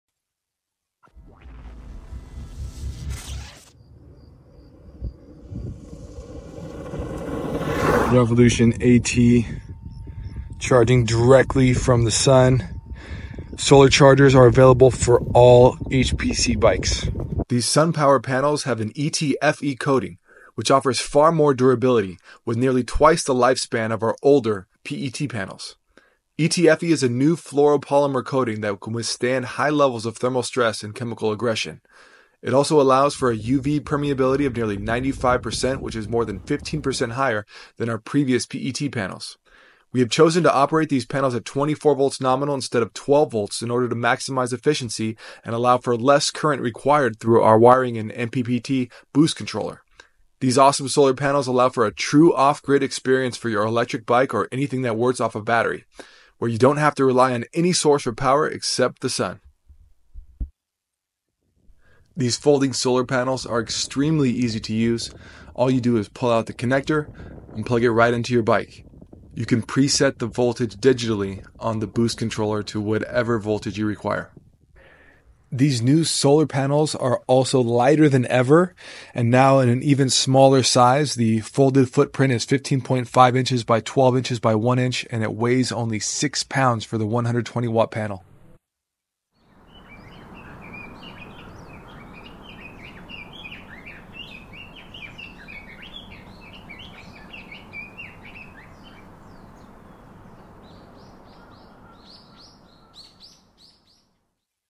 Can you charge your E-bike with a solar panel? This is an overlander's dream!
Overview of new 2023 HPC high performance folding panels to charge ANY voltage battery system from 36V to 84V nominal!
We offer small, portable 90W, 120W, 180W, and 300W solar panels at 24V nominal output with solar converters to charge 36V, 48V, 52V, 63V, 74V, 86V battery systems.

Bike featured in video is SOLD OUT Revolution AT which was a special run of an insane 9.5" Travel Full Suspension Fat bike with 4.8" tires!

**ESTIMATED CHARGE TIMES from fully depleted battery**

500Wh battery/ 300W panel = around 1.75 hours in perfect sunlight
1000Wh battery/300W panel - around 3.5 hours in perfect sunlight

500Wh battery/180W panel = Around 2.8 hours in perfect sunlight
1000Wh battery/180W panel = Around 5.6 hours in perfect sunlight